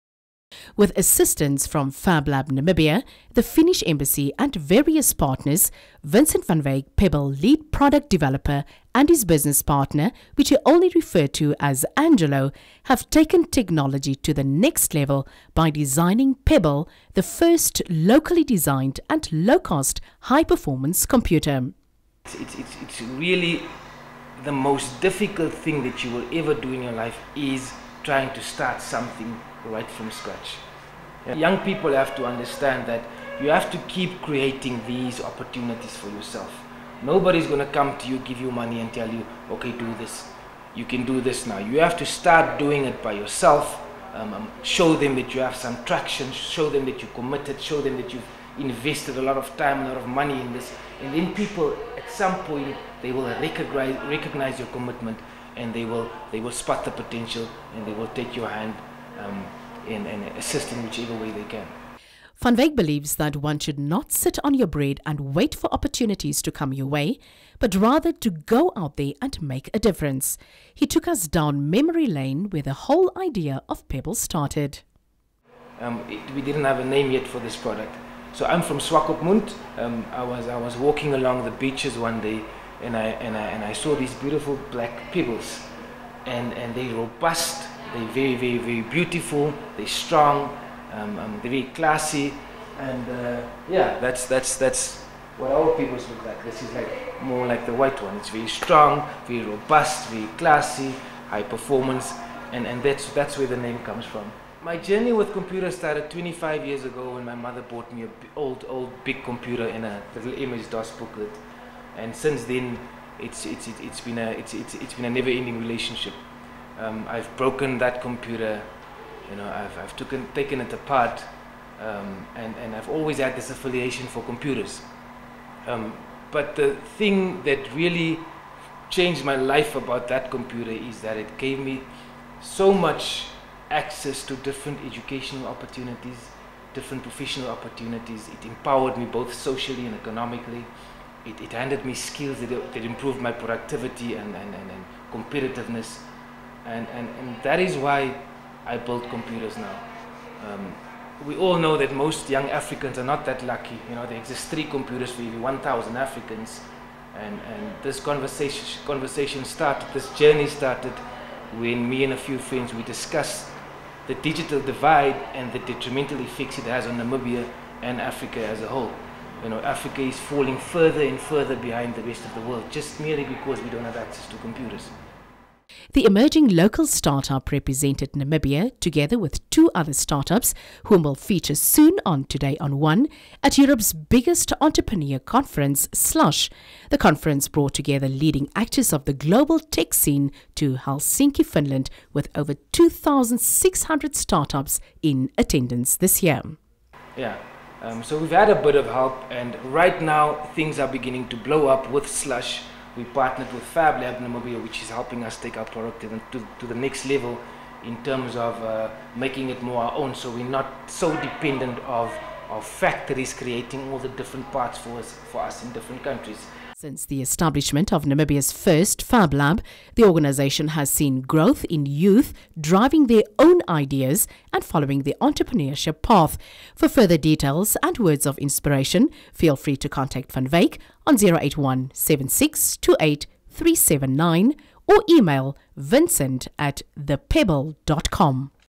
PEBL - Namibia’s very own low cost, high-performance computer. By: Hendrina Kanyolo
Who would have thought that a pebble on the beach would ignite an idea so inspirational, that might change the digital future of the African Continent? With assistance from FABlab Namibia, the Finnish Embassy and various partners, Vincent van Wyk, PEBL Lead Product Developer and his business partner which he only referred to at Angelo, have taken technology to the next level by designing PEBL, the first locally designed and low-cost high-performance computer. Van Wyk believes that one should not “sit on your bread” and wait for opportunities to come your way, but rather to go out there and make a difference. He took us down memory lane where the whole idea of PEBL started. The emerging local startup, represented Namibia, together with two other startups, whom will feature soon on Today on One, at Europe’s biggest entrepreneur conference, SLUSH. The conference brought together leading actors of the global tech scene to Helsinki Finland with over 2600 startups in attendance this year. Since the establishment of Namibia’s first FABlab, the organization has seen growth in youth driving their own ideas and following the entrepreneurship path.